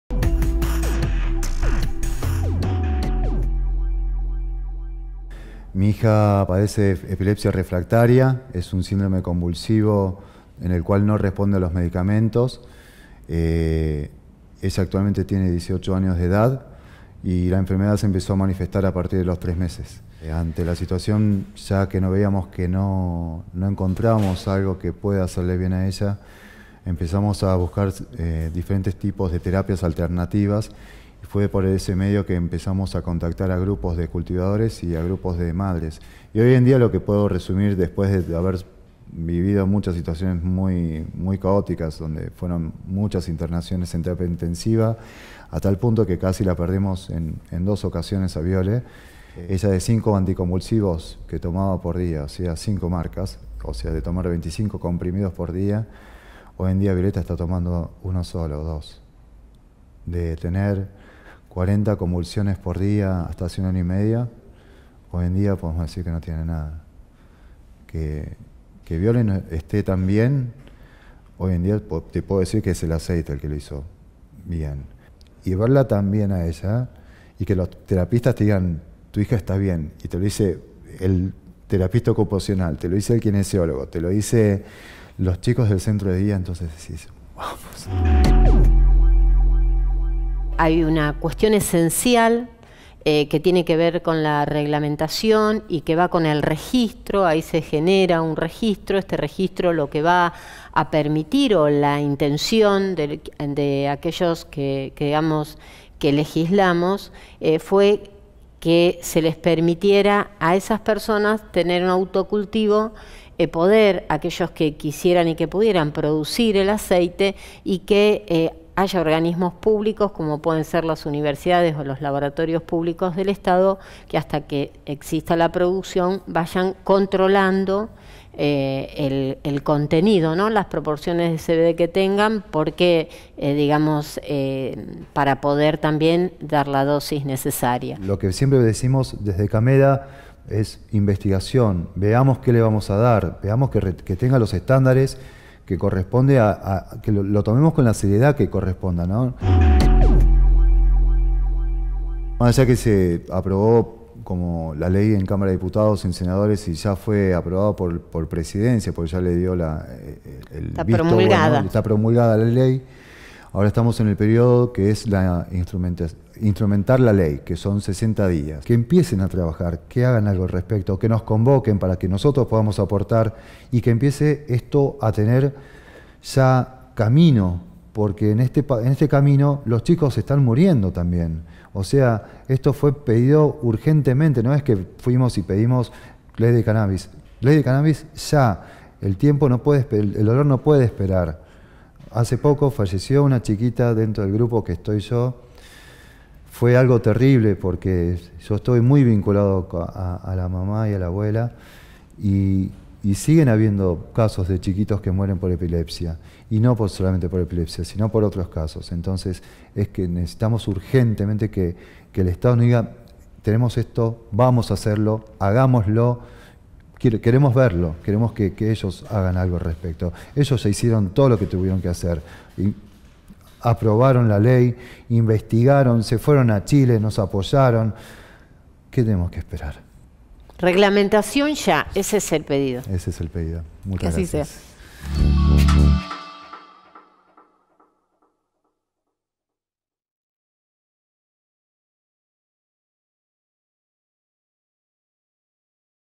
Cannabis medicinal: la reglamentación que no llega
En su primera sesión de 2017, el Senado dio la media sanción que faltaba para convertir en ley el uso del cannabis medicinal en todo el territorio nacional. Este 17 de julio vence el plazo para la reglamentación, que permitiría la puesta en práctica de la ley. En otras palabras, la fabricación en laboratorios públicos del aceite cannábico -recomendado en tratamientos de enfermedades como epilepsia refractaria, Parkinson, VIH, cáncer, esclerosis, autismo, y artrosis, entre otras- y su comercialización a un costo accesible.

La diputada Gabriela Troiano, cofirmante del proyecto de ley, y Juan Peñalba, papá de Violeta y miembro de CAMEDA (Cannabis Medicinal Argentina), explican los beneficios que el tratamiento opera en los pacientes y la urgencia de que la norma sea puesta en práctica.